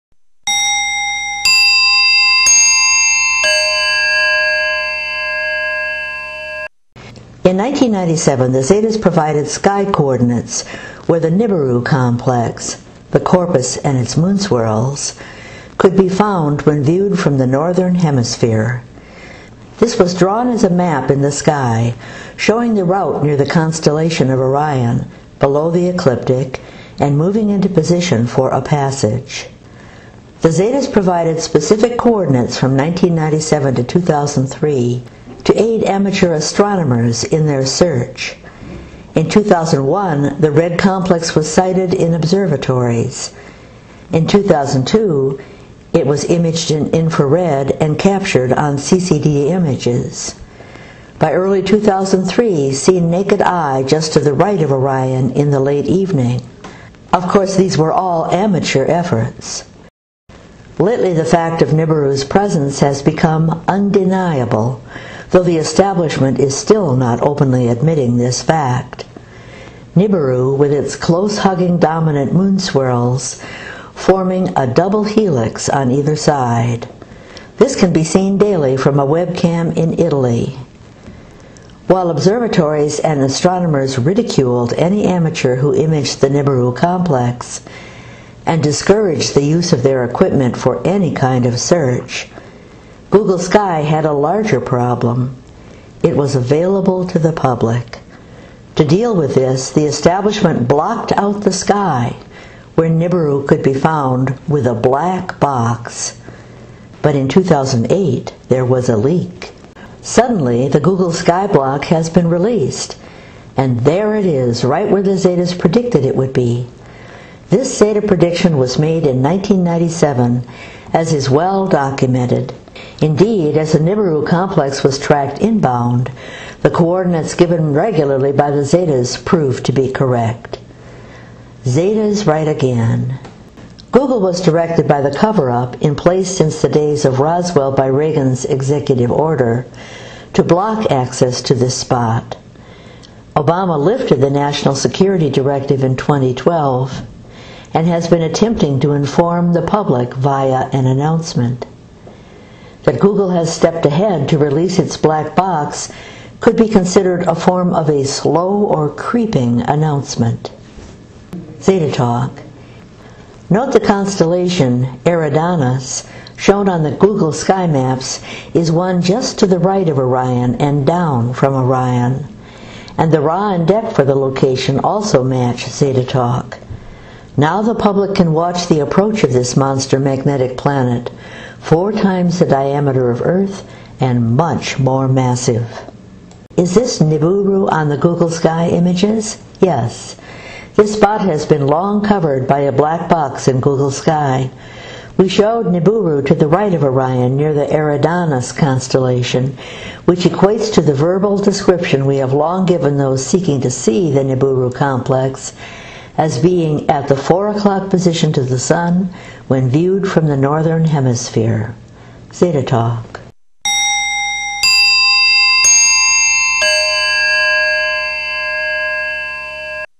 Zeta Report Google Sky Nibiru
In 1997 ZetaTalk provided coordinates, RA and Dec in the sky, for the inbound Nibiru complex. Nibiru was tracked along this path from 2001 to 2003, and now is found on Google Sky at the very RA and Dec predicted 18 years ago! Zetas RIGHT again!